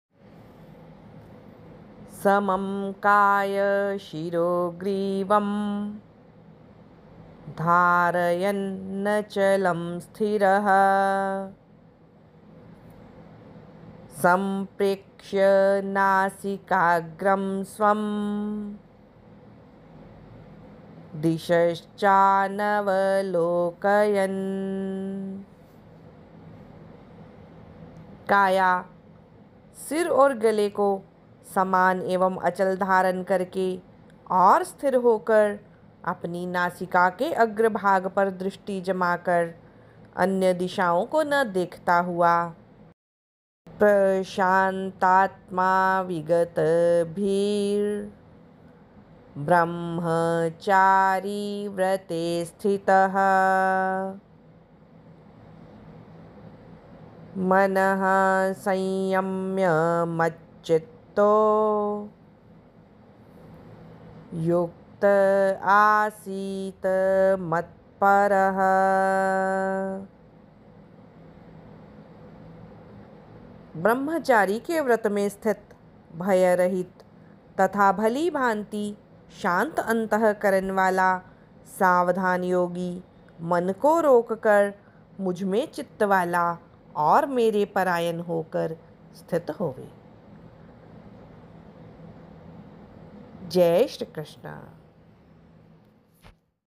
श्रीमद्भगवद् गीता || नित्य गीता पाठ (अध्याय-06 श्लोक-13 व 14) || Bhagwad Geeta Shalok
समं कायशिरोग्रीवं धारयन्नचलं स्थिरः।
सम्प्रेक्ष्य नासिकाग्रं स्वं दिशश्चानवलोकयन्‌॥

भावार्थ
काया, सिर और गले को समान एवं अचल धारण करके और स्थिर होकर, अपनी नासिका के अग्रभाग पर दृष्टि जमाकर, अन्य दिशाओं को न देखता हुआ॥13॥

प्रशान्तात्मा विगतभीर्ब्रह्मचारिव्रते स्थितः।
मनः संयम्य मच्चित्तो युक्त आसीत मत्परः॥

भावार्थ
ब्रह्मचारी के व्रत में स्थित, भयरहित तथा भलीभाँति शांत अन्तःकरण वाला सावधान योगी मन को रोककर मुझमें चित्तवाला और मेरे परायण होकर स्थित होए॥14॥